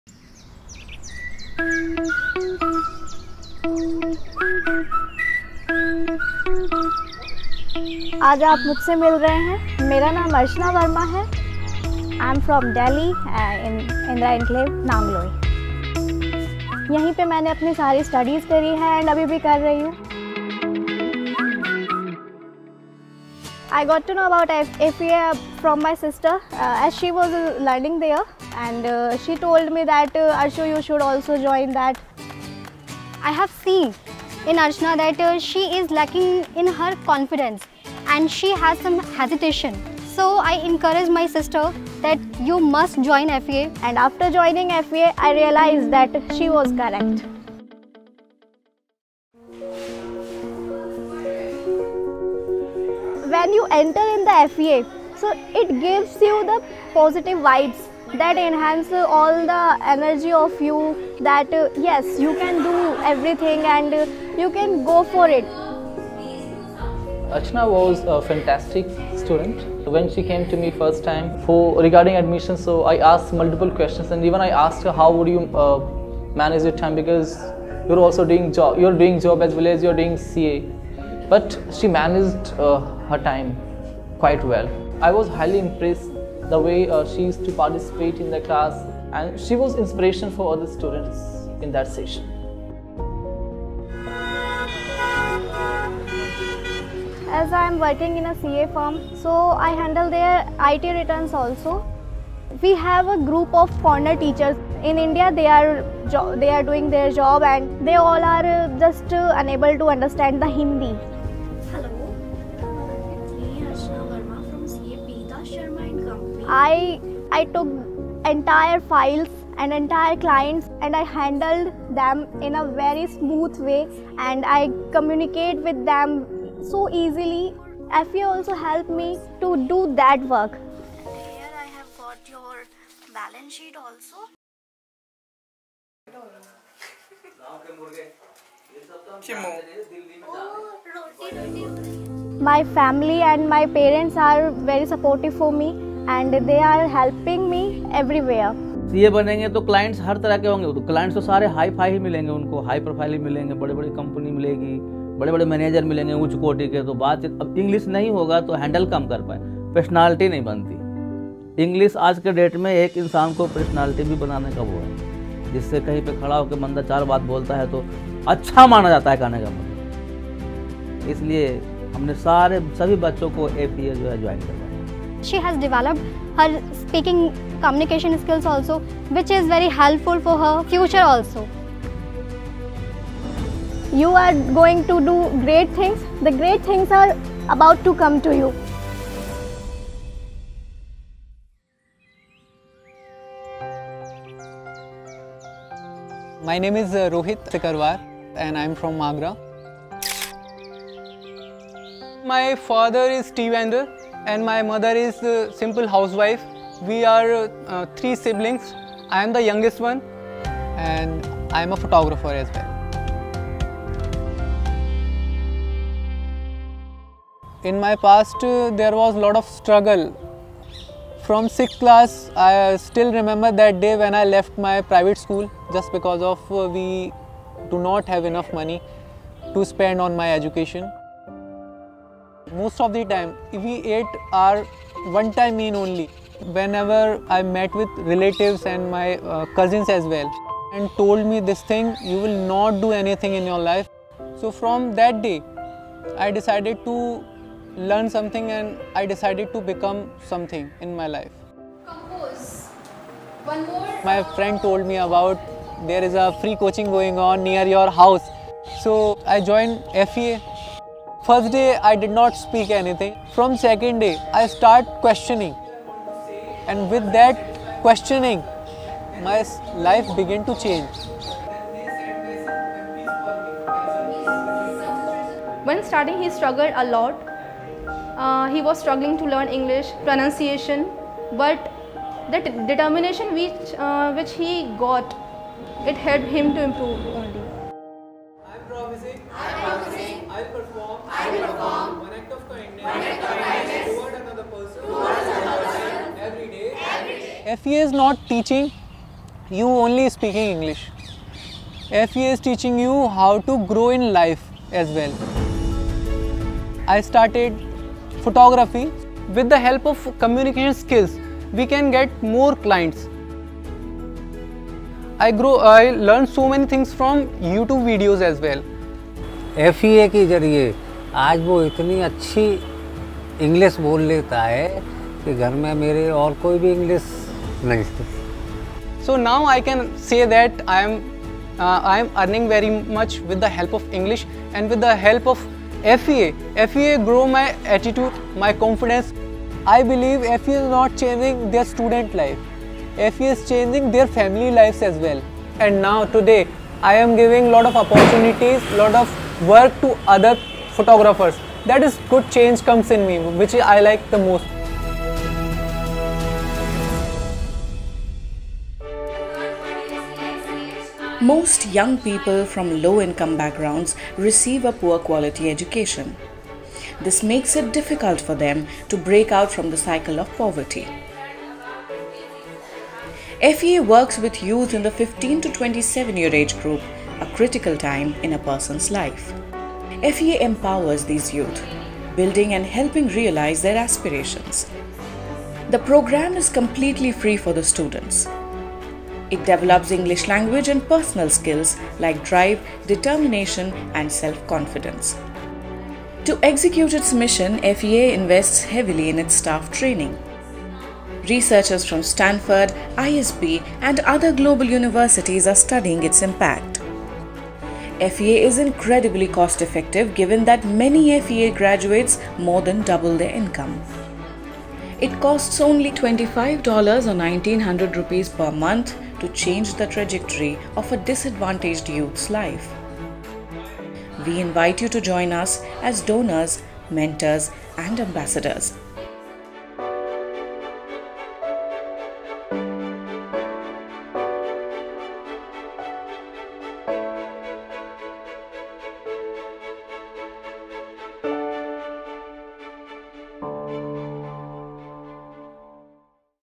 Freedom Employability Academy (FEA) - Stories
Explore FEA's work through Archana and Rohit's stories.